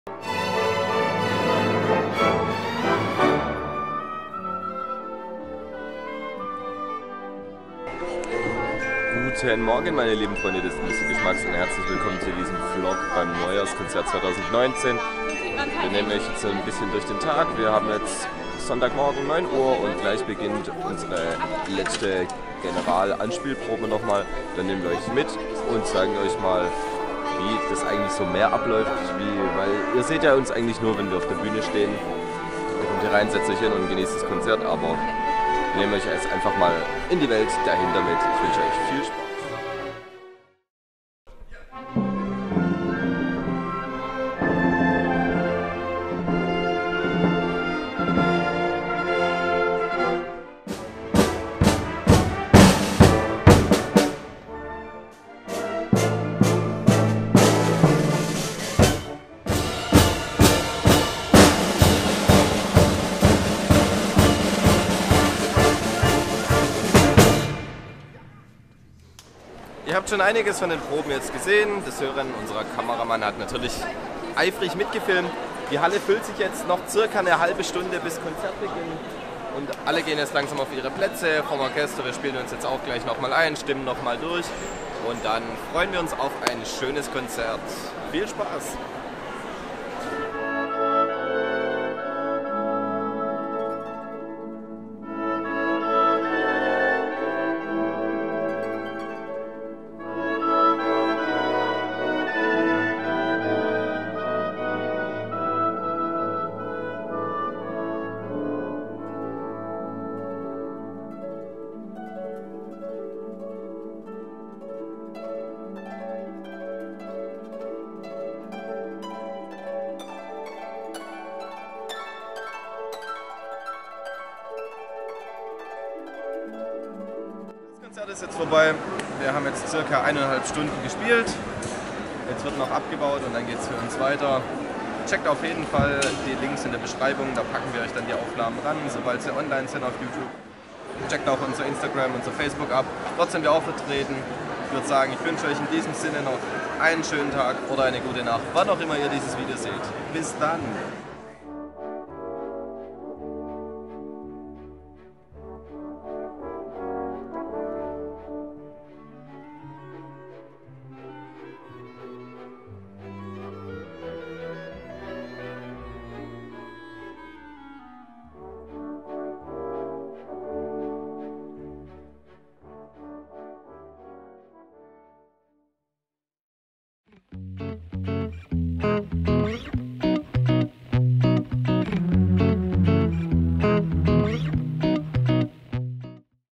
Jugendsinfoieorchester Neckarsulm -Neujahrskonzert 2019 |Vlog
Hallo zusammen wir sind das Jugendsinfonieorchester Neckarsulm und in diesem Vlog nehmen wir euch hinter die Kulissen des Neujahrskonzertes mit.
Moderator: Lukas Baumann
Kamera: Sören Friedel
Schnitt: Sören Friedel
Foto Thumbnail: Sebastian Erlewein
Eine Sandc-Pictures Produktion im Auftrag vom
Jugendsinfonieorchester Neckarsulm!